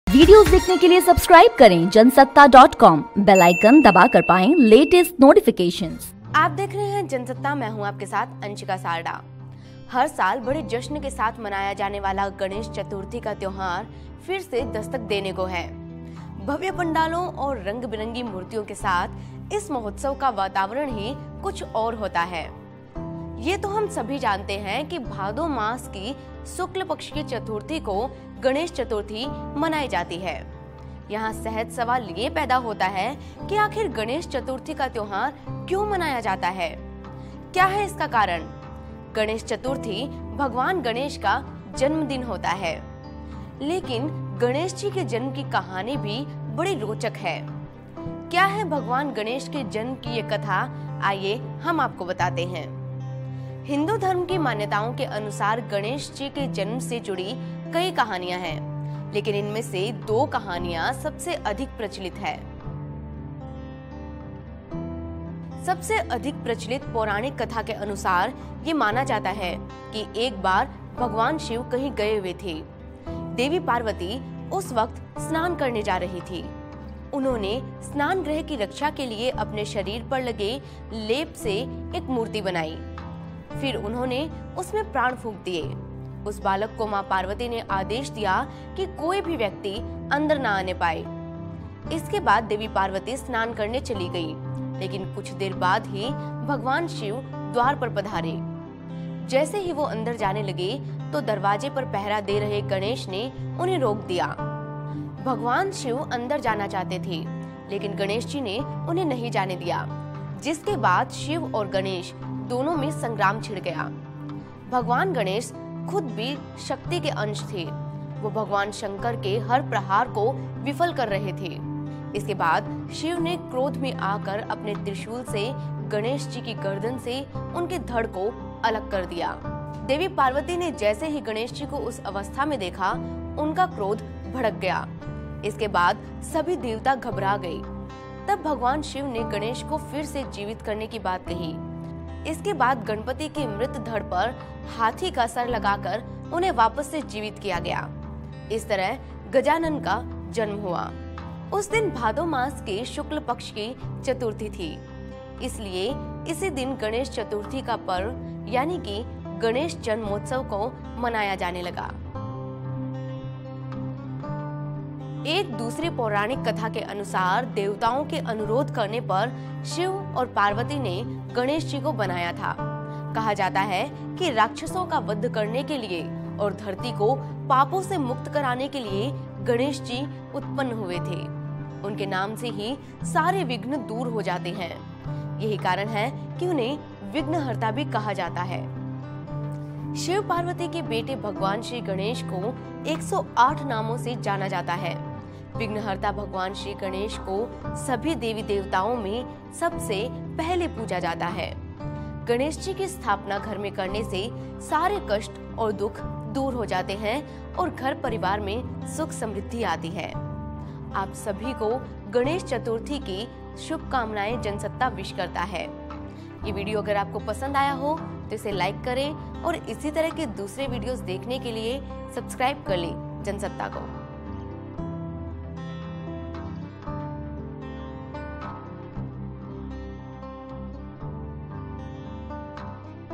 Ganesh Chaturthi: गणेश चतुर्थी मनाने के पीछे की पौराणिक कथा
ganesh chaturthi: कथा के अनुसार माना जाता है कि एक बार भगवान शिव कहीं गए हुए थे.देवी पार्वती उसी वक्त स्नान करने जा रहीं थीं.तो उन्होंने स्नानगृह की रक्षा के लिए अपने शरीर पर लगे हुए चंदन लेप से एक मूर्ति बनाई और फिर उसमें प्राण डाल दिए. उस बालक को पार्वती ने आदेश दे दिया कि कोई भी आए तो उसे अंदर न आने दे. इसके बाद देवी पार्वती स्नान करने चलीं गईं.कुछ देर बाद भगवान शिव द्वार पर आते हैं. जैसे वो अंदर जाने लगते हैं तो दरवाजे पर पहरा दे रहे गणेश उन्हें रोक देते हैं.भगवान शिव ने गणेश से अंदर जाने की बात कही, लेकिन गणेश ने उन्हें अंदर नहीं जाने दिया. जिसके बाद शिव और गणेश में संग्राम छिड़ गया. पूरी कथा जानने के लिए देखिए ये वीडियो...